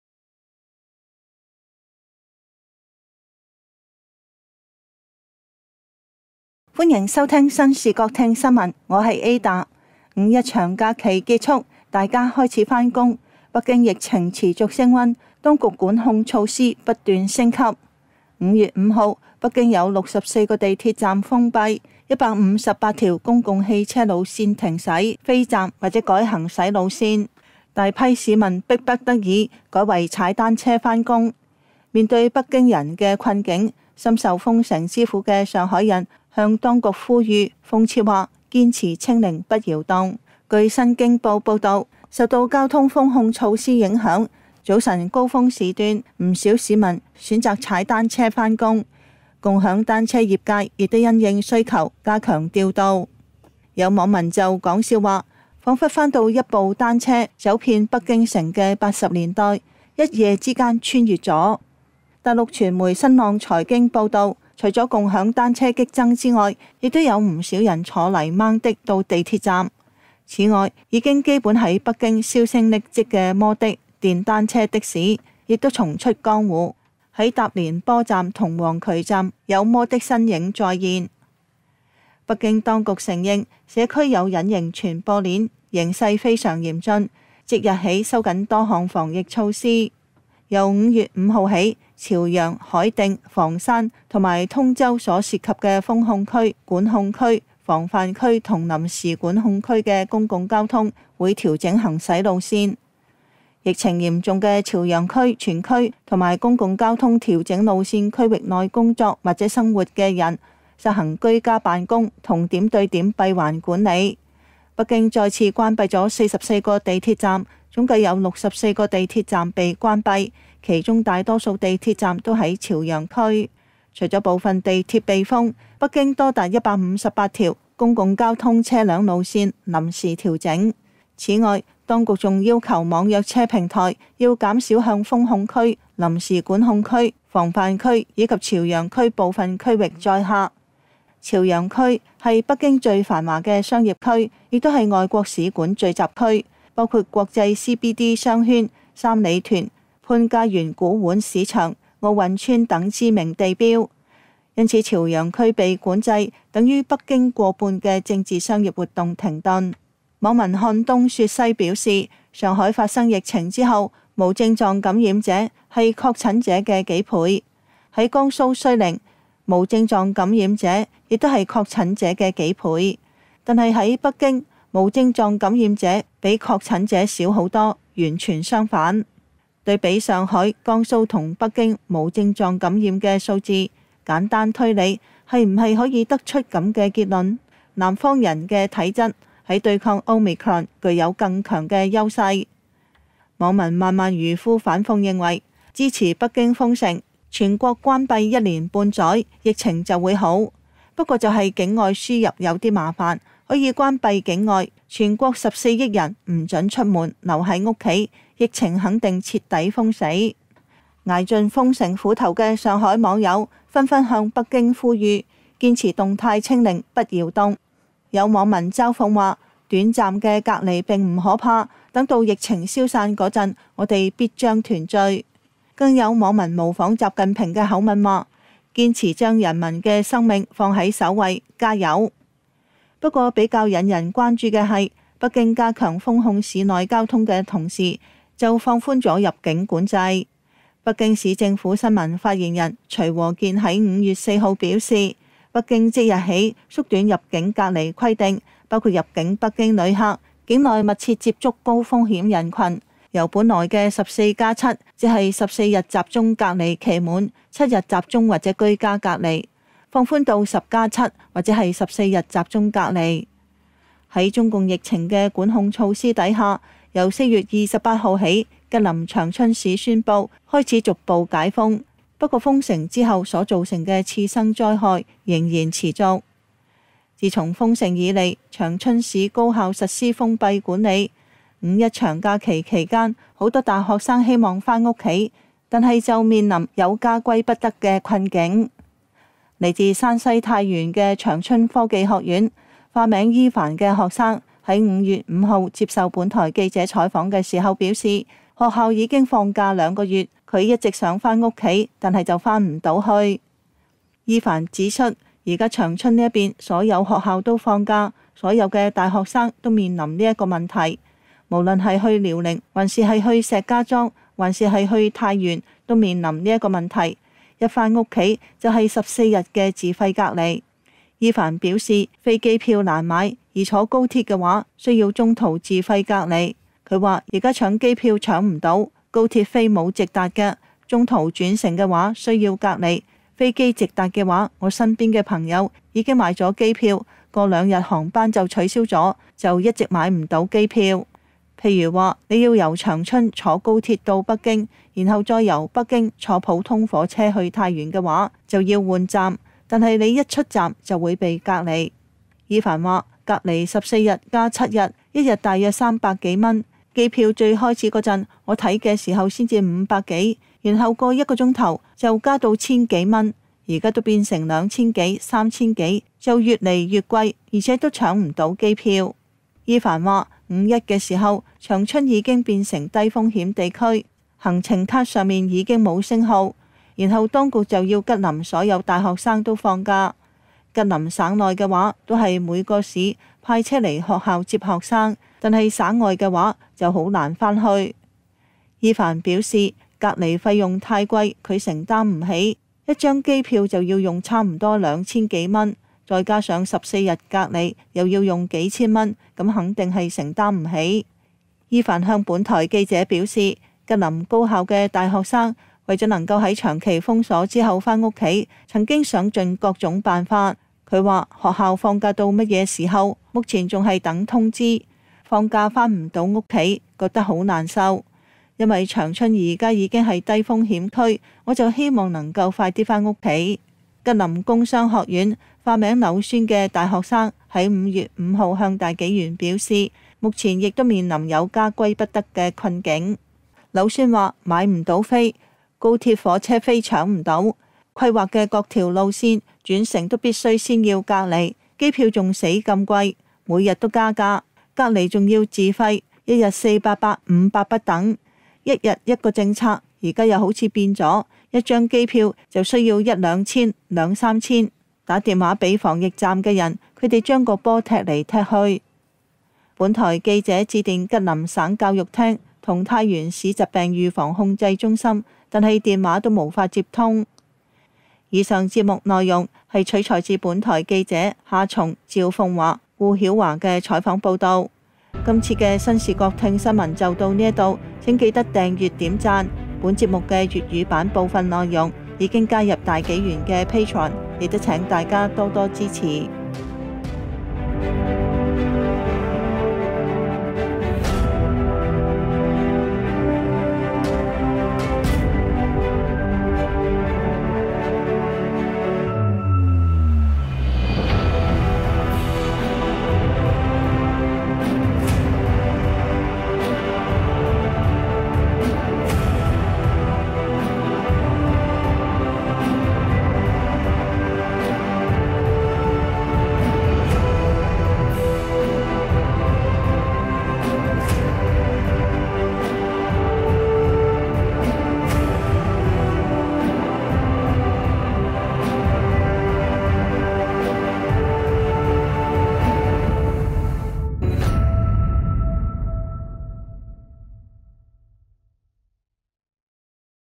清零升級 北京地鐵封 吉林大學生回家難 ​
【新視角聽新聞】中國疫情持續升溫，當局管控措施不斷升級。面對北京人的困境，向當局喊話反諷，「堅持清零不動搖」。深受封城之苦的長春大學生面臨有家難回的窘境。

新紀元4月刊【烏克蘭 史詩級戰爭】：📖現已上市🎉